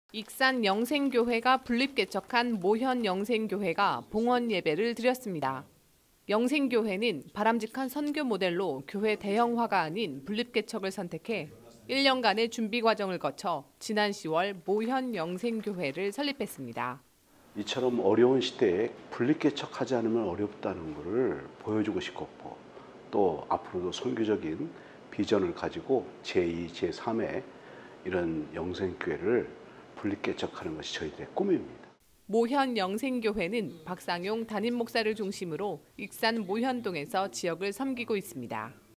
전북 익산영생교회 50주년 기념 분립개척 모현영생교회 봉헌예배 l CTS뉴스 l CTS뉴스
익산 영생교회에서 분립·개척한 모현영생교회가 봉헌예배를 드렸습니다.

영생교회는 바람직한 선교 모델로 교회 대형화가 아닌 분립 개척을 선택해 1년 간의 준비과정을 거쳐 지난 10월 모현영생교회를 설립했습니다.

INT 박용호 감독 / 기독교대한감리회 호남특별연회
이처럼 어려운 시대에 분립 개척하지 않으면 어렵다는 것을 보여주고 싶고 앞으로 선교적인 비전을 가지고 제2, 3의 영생교회를 분립개척하는 것이 저희들의 꿈입니다

모현영생교회는  박상용 담임목사를 중심으로 익산 모현동에서 지역을 섬기고 있습니다.